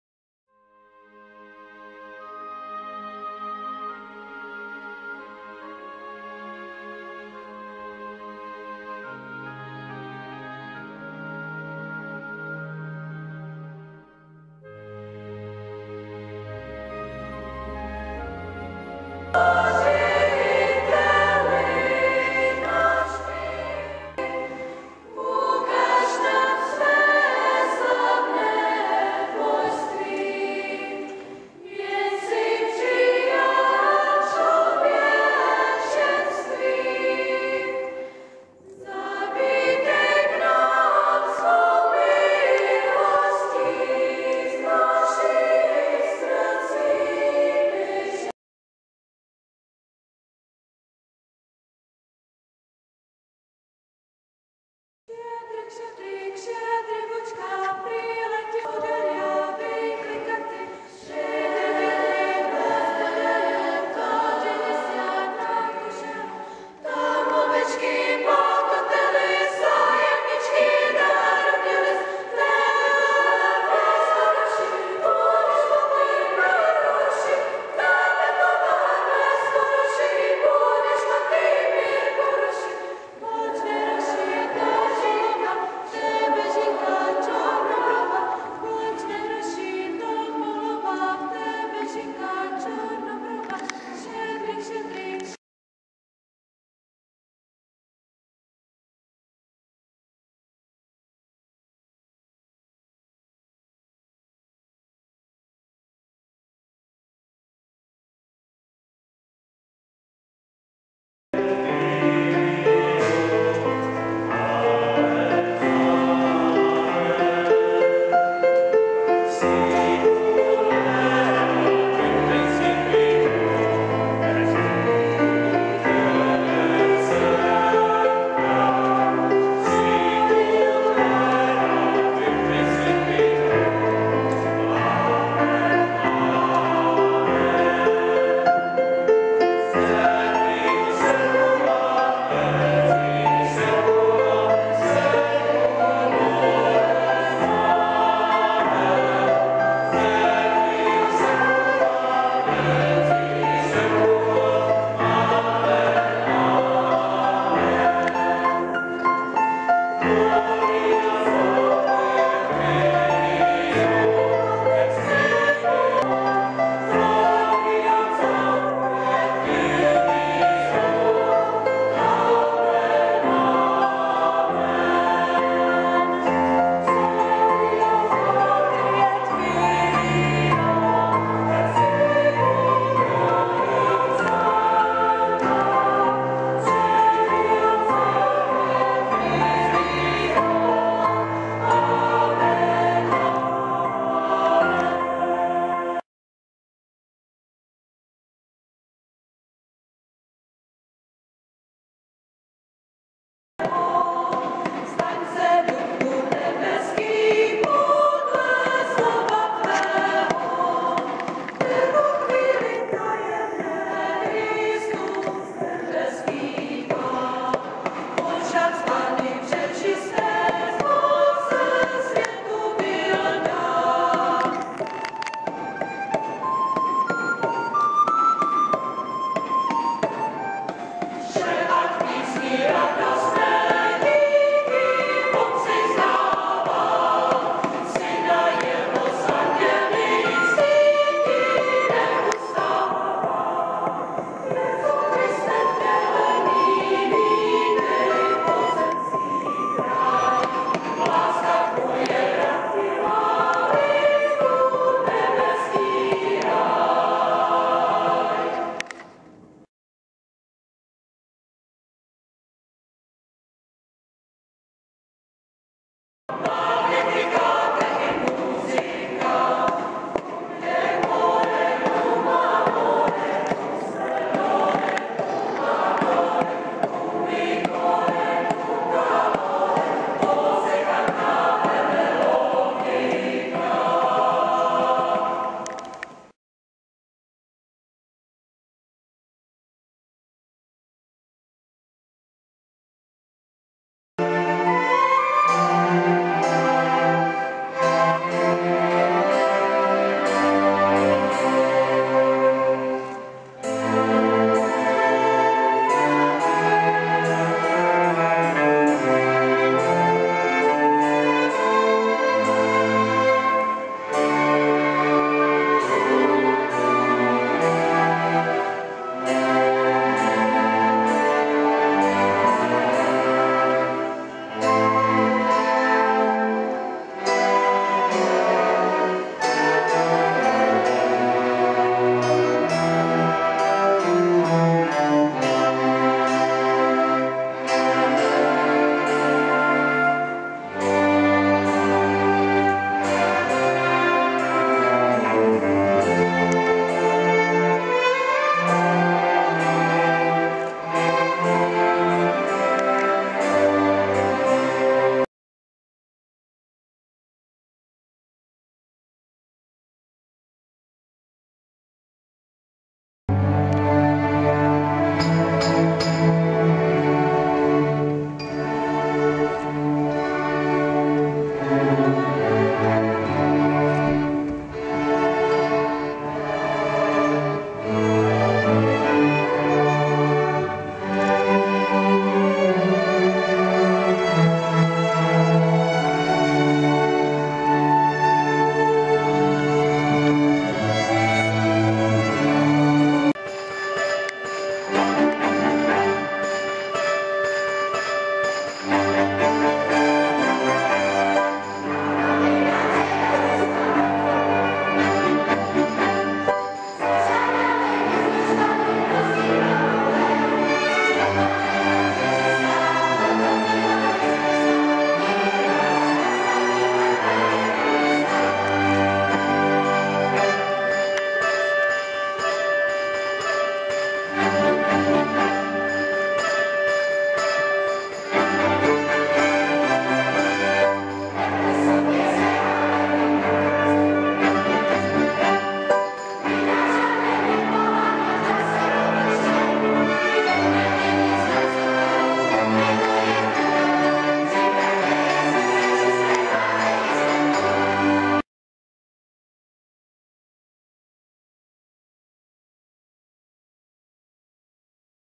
Vysoké Mýto Adventní koncert 15.12.2013 část II.
Adventní koncert chrám sv. Vavřince Vysoké  Mýto   z ukázky ZUŠ Vysoké Mýto            a smíšeného pěveckého sboru Otakar  Vysoké Mýto